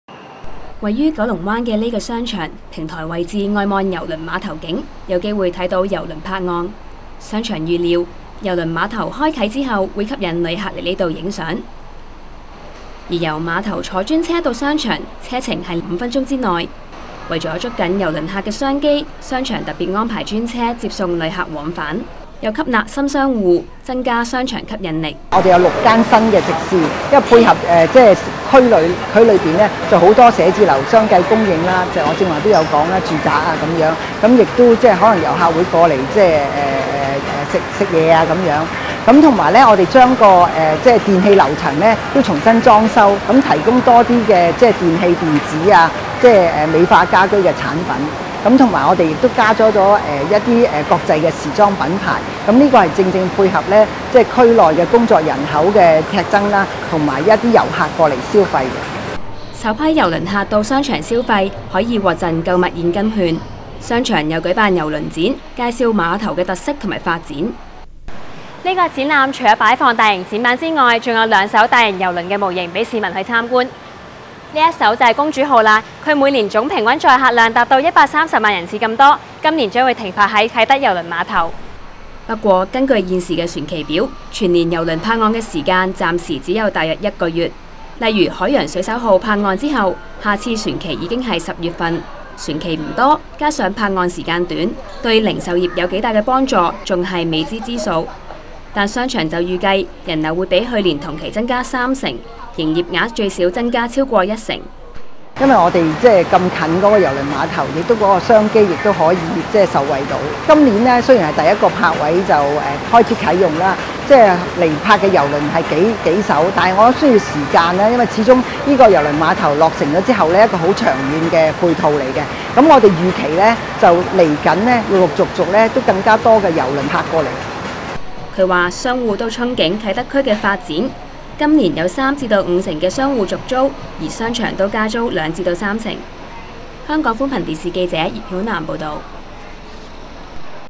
啟德郵輪碼頭啟用 商場準備就緒
啟德郵輪碼頭首日啟用第一艘泊岸的「海洋水手號」，會接載超過三千名乘客，為了迎接首批郵輪客，九龍東各個商場安排免費專車接送往返，有商場吸納新商戶迎接新客源，估計可以令營業額增加超過一成，新簽的租約就加租兩至三成。